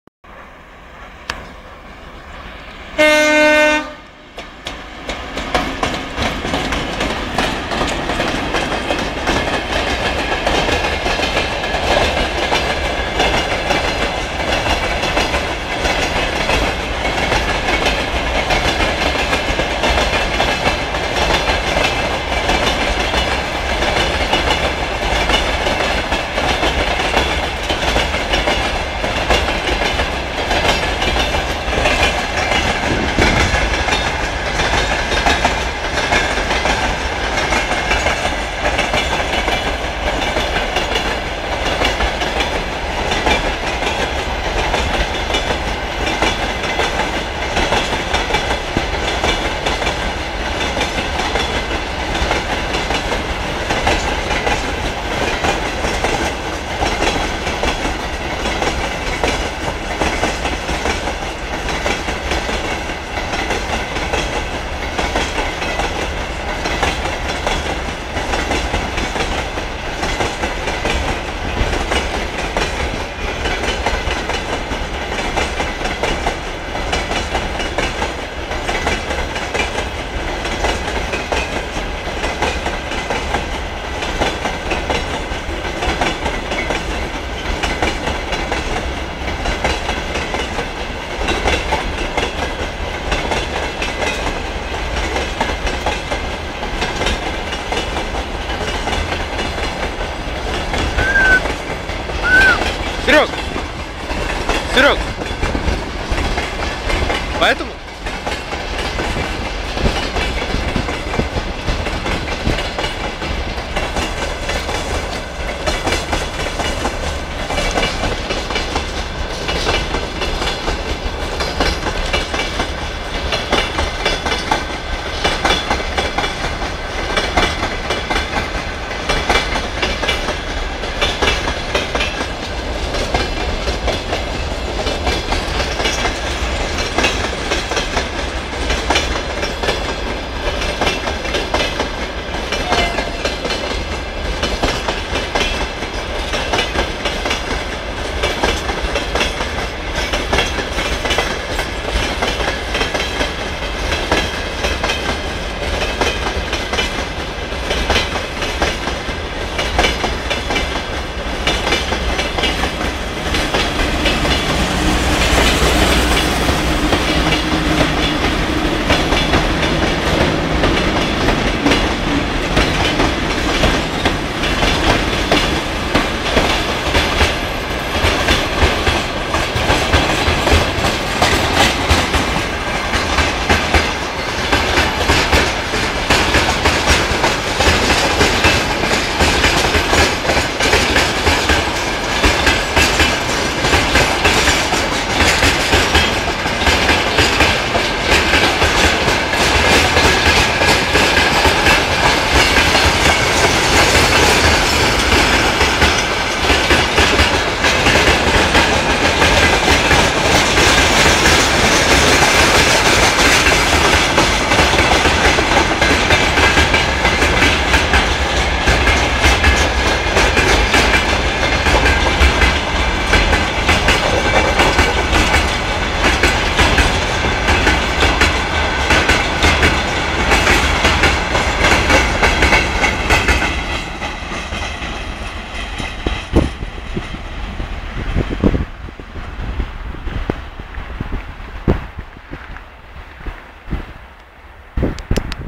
ВЛ10у с грузовым + ВЛ10к-315 с грузовым,БМО
Перегон Кубинка-2 - пл.211 км